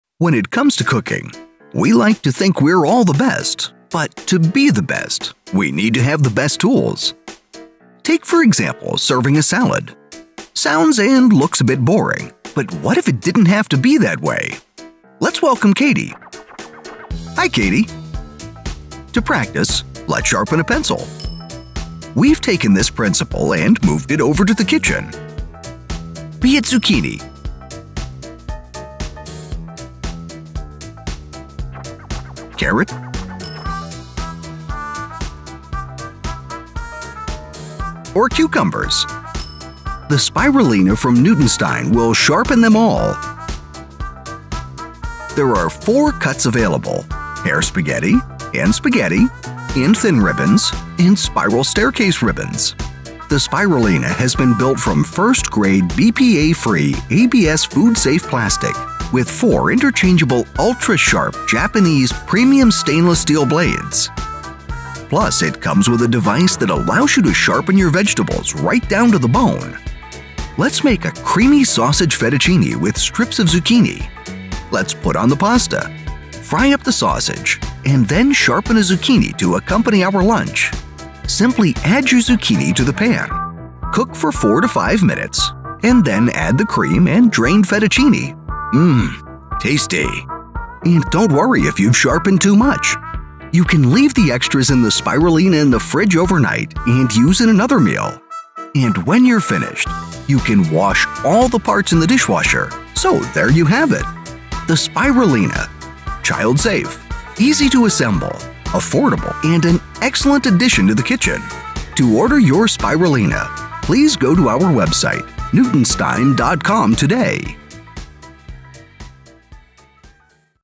Spiralina - Vegetable Spiraliser - 4 Blades to create Spaghetti or Ribbon swirls of vegetables!
SPIRALINA - VEGETABLE SPIRALISER - 4 SETTING TOOL, SPAGHETTI / JULIENNE AND SPIRAL CUTTER OF VEGETABLES FOR STIR-FRIES, SALADS OR PASTA DISHES + HAND GUARD + RECIPE BOOK AND MANUAL

With Spiralina make endless courgetti slices julienne/spaghetti like strips of vegetables and swirls like round stair cases ideal for a gluten-free / low-carb / raw food / paleo / vegetarian / vegan diet or even just as a creative cooking tool.

Sturdy design with 4 intechangeable razor sharp japanese stainless steel blades (In a special secure no hand cuts design). The spiralizer can create two styles of slices from one tool. Two blades to create thin or thick julienne/spaghetti like strips and two blades to create thin or thick spiral stair case like swirls or roses of fresh vegetables.

The Spiral/Spiralizer Slicer enables you conjure up endless Julienne/spaghetti strips and swirls of carrot zucchini radish cucumber rutabagas turnips sweet potatoes squash and all kinds of other firm vegetables.

Compact and Easy to use with large storage container. This tool is easy to use clean and stow away.
Comes with 2 spaghetti like cutter blades 3mm and 6mm and 2 spiral staircase ribbon like cutter 3mm and 6mm.

NEXT GENERATION SLICER BIGGER LESS WASTAGE: This slicer is bigger and more durable than other traditional slicers bigger size means it can accommodate vegetables up to 5.5 CM in diameter compared to only 4 - 4.5 CM in the normal spiralizer. 50% less vegetable wastage due to the new deep-penetrating cap design. Use a simple turning motion (just like sharpening a pencil) to instantly turn boring vegetable into irresistible veggie spirals. User friendly and Ergonomic Design
Non-toxic: Built from first-grade BPA-free ABS food-safe plastic and 4 ultra-sharp Japanese premium stainless steel blades guaranteed not to rust or lose sharpness over time. Super easy to use & clean.

A cooking enthusiasts dream to prepare fruits or vegetables in extraordinary presentation cooked or uncooked. This is Newtonstein's latest design protected and patented kitchen innovation.
Dimensions:  7.7cm Diameter x 18.8cm height.
How to Use:

- Select one of four blade tool inserts and place it in the slot of the tool cover.

- Place the spiralising tool ontop of the storage container twist to lock.

- Hold a firm vegetable like a carrot courgette cucumber or even aubergine vertically over the blade V groove and push down until it is slotted in.

- Rotate the vegetable clockwise to begin pushing it through the blades like a pencil sharpener and that is it!

Within seconds obtain spaghetti like cuttings or endless spirals of ribbons depending on your choice of blade including whether you chose the thin or thick slicer.
Contents:

- 1 Spiralina tool

- 1 Large storage container

- 4 Spiral blade inserts 2 spaghetti blades (3mm & 6mm) 2 spiral blades (3mm & 6mm)

- 1 Hand guard tool

- Recipe booklet

- Instruction Manual (5 languages EN DE FR ES IT)